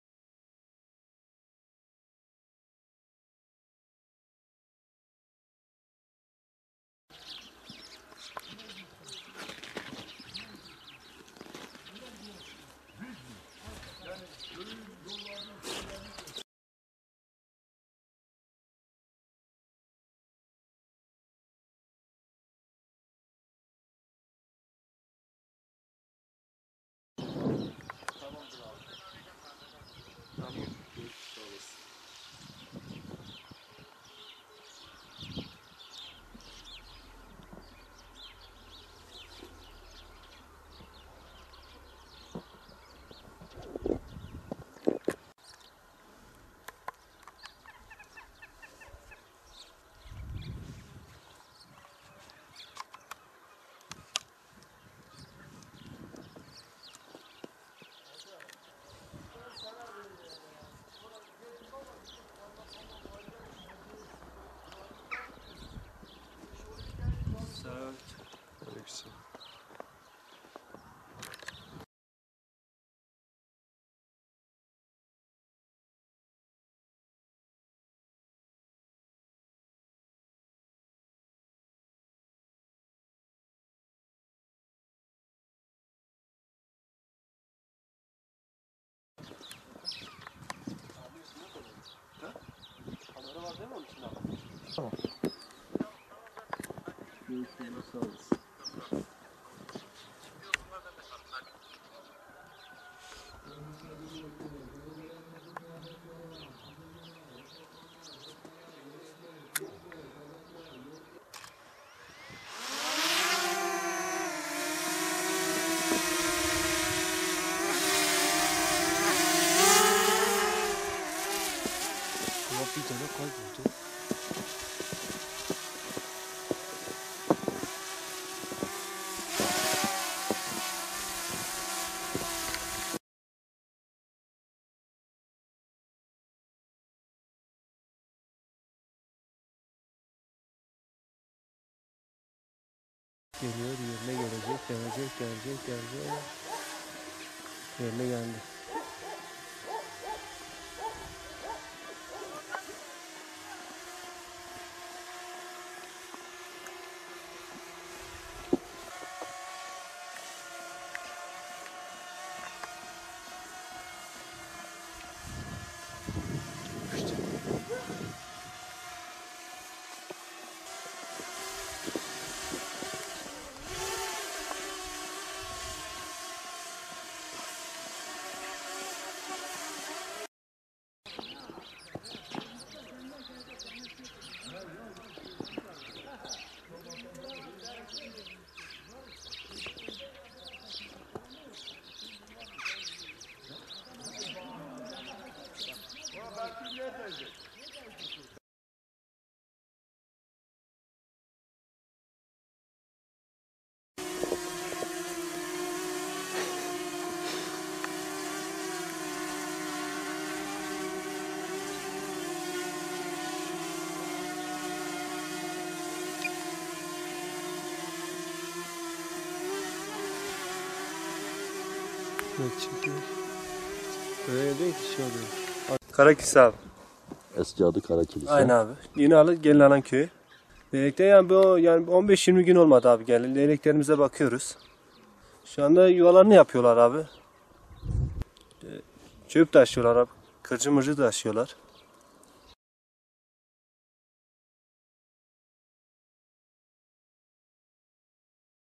Kars'a dönen leylekler, yuvalarını yeniliyor
Havaların ısınmasıyla birlikte Kars'a geri dönen leylekler, kar yağışı ve rüzgarın dağıttığı yuvalarını yenilemeye başladı. Her yıl baharla birlikte Kars ve çevresini mesken tutan leylekler, havaların soğumasıyla birlikte göç ettikleri sıcak bölgelerden geri dönmeye başladı. Selim ilçesine bağlı Gelinalan köyündeki elektrik direkleri üzerlerindeki yuvalarına yerleşen leylekler, bir taraftan da kar yağışı ve rüzgarın bozduğu bölümleri onarıyor. İçerisini temizledikleri yuvalarındaki çalı çırpıyı uzun gagalarıyla temizleyen leylekler, araziden topladıkları dalları da büyük bir dikkatle taşıdı. Gelinalan köyü sakinleri, leyleklerin yuvalarını onarmasını büyük bir dikkatle izledi.